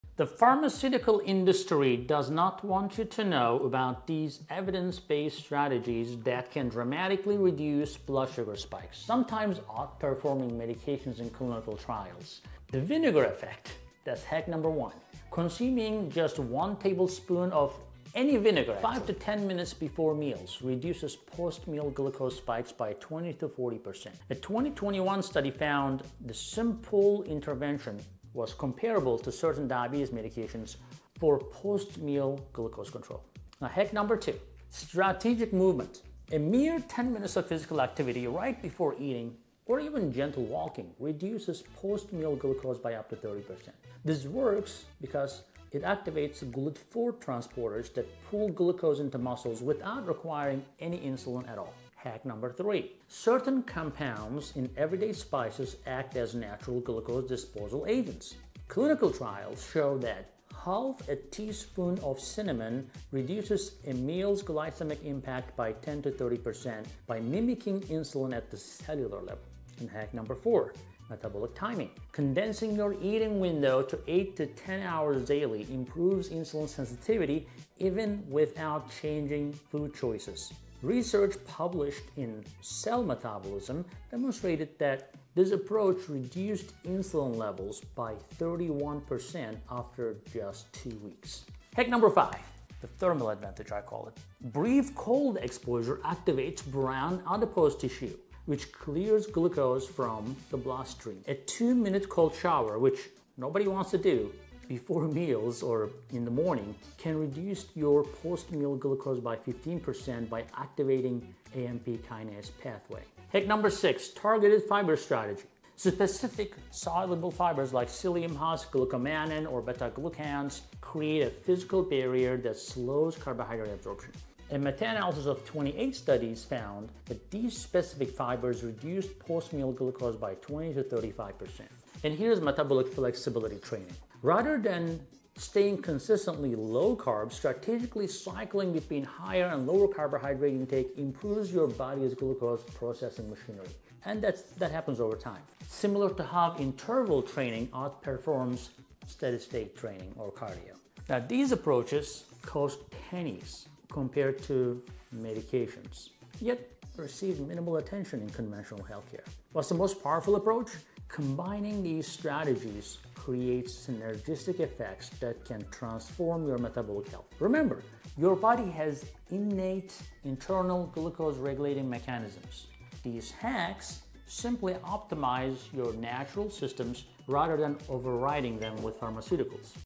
7 Blood Sugar Hacks Big Pharma Hopes You Never Find Out!
Discover 7 science-backed ways to lower blood sugar spikes naturally — some more effective than medications in studies! From vinegar and cinnamon to cold exposure and strategic movement, these hacks optimize your body’s glucose control without relying on drugs. 💥